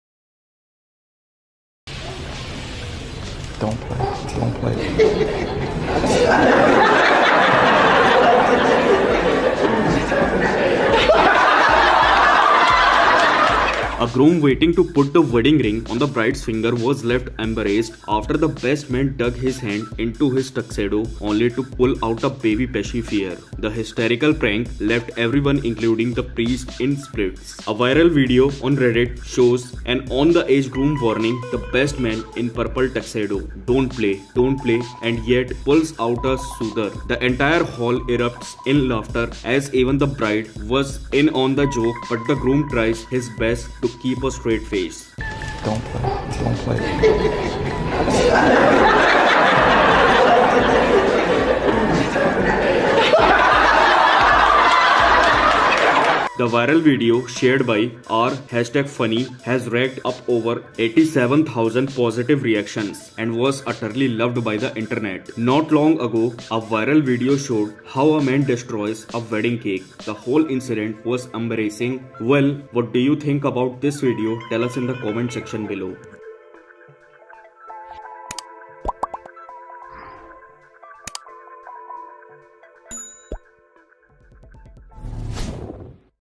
Groom left embarrassed as best man gives him baby soother instead of wedding ring | Viral Video
About Video :-
A groom waiting to put the wedding ring on the bride’s finger was left embarrassed after the best man dug his hand into his tuxedo only to pull out a baby pacifier. The hysterical prank left everyone, including the priest, in splits.

Viral videos,
Viral video,